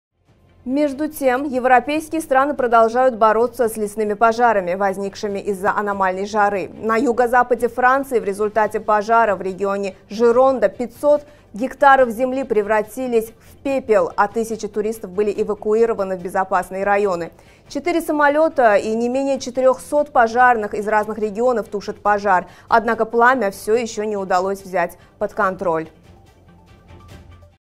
На Юго-Западе Франции не Удаётся Потушить Пожар | Baku TV | RU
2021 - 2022 ©️ Baku TV Ru ©️ Baku TV